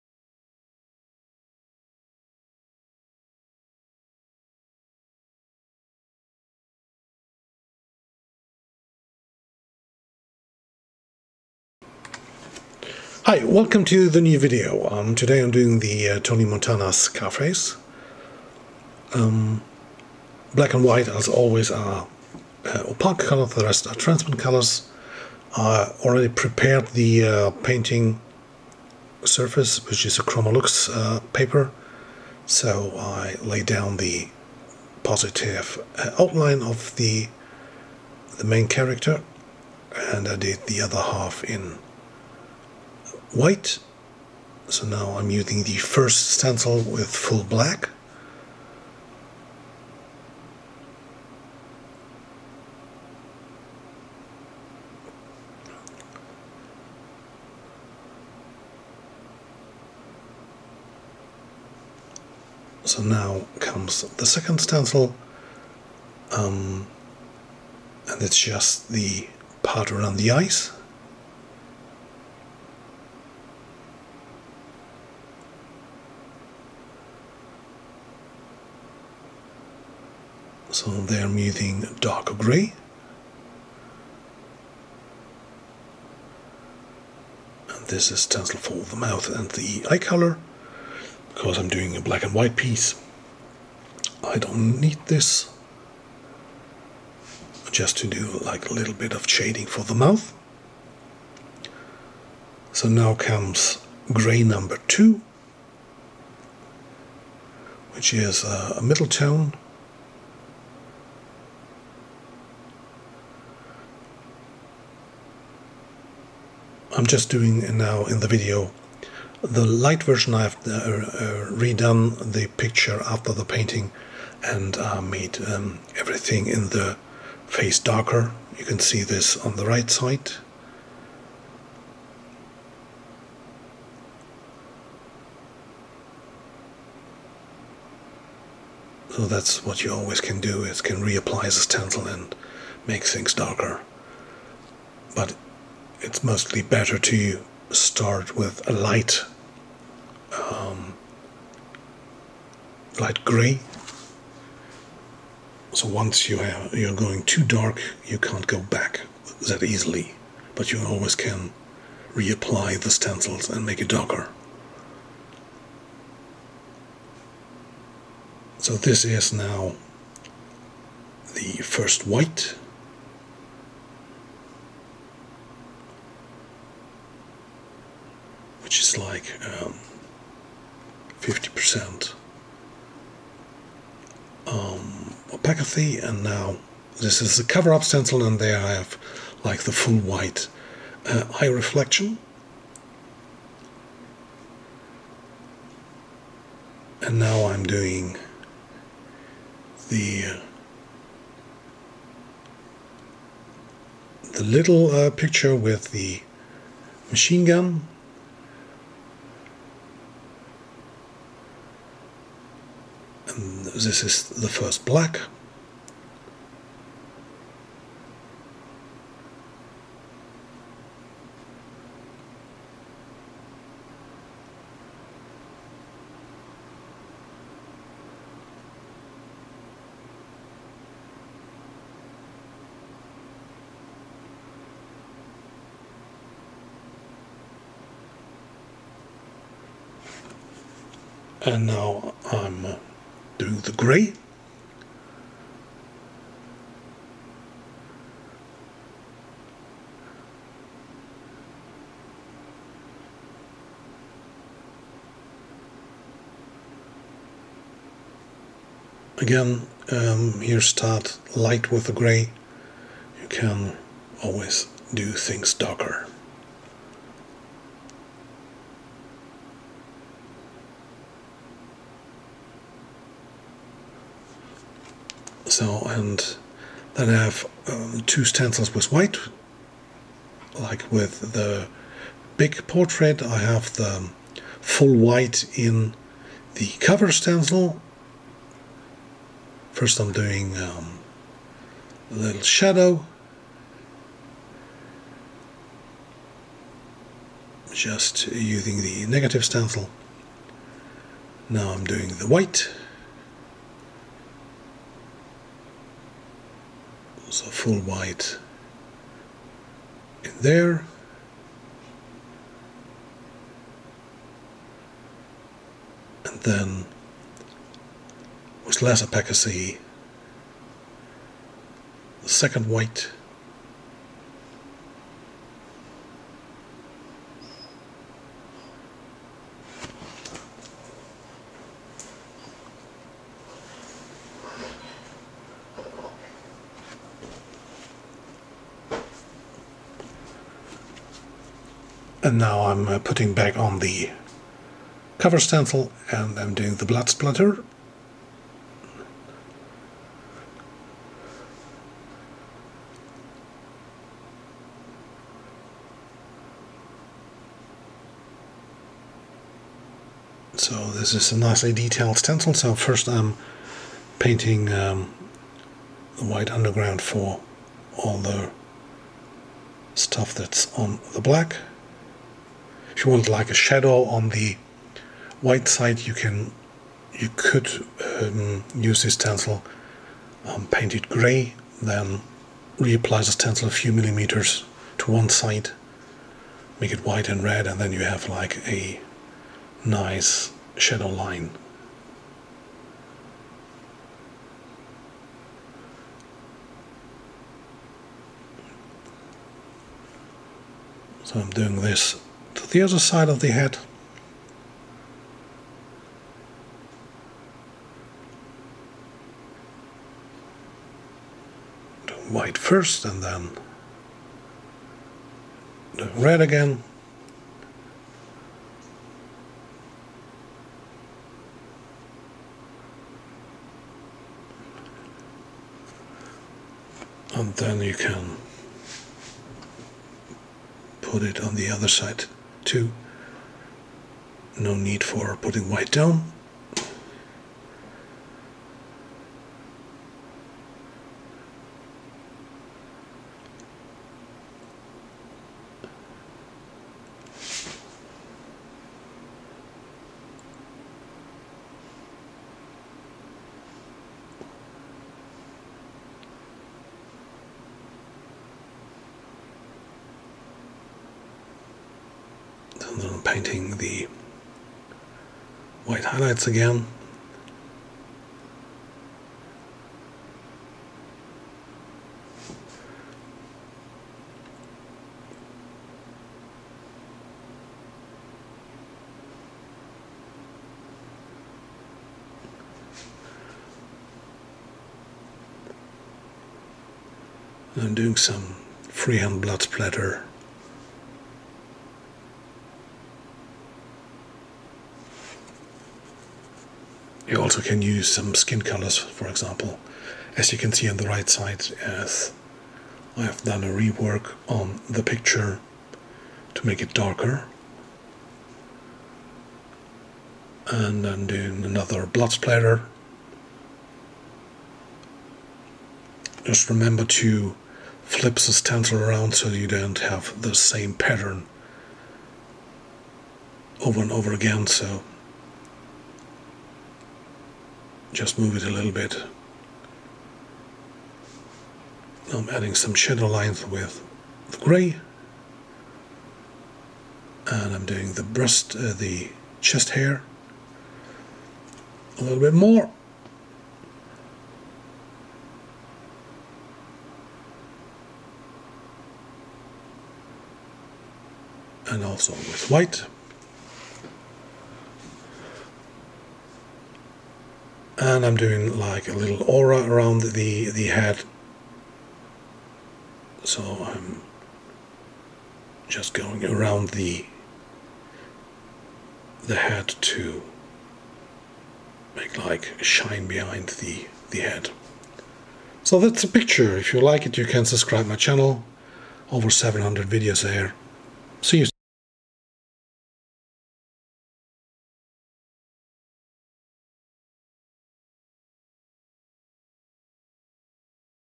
Airbrush by Wow No.733 " Scarface " english commentary
for questions watch the text in the " About " section under the Video
Airbrush with the Stencil technique for the perfect and fast picture of Scarface .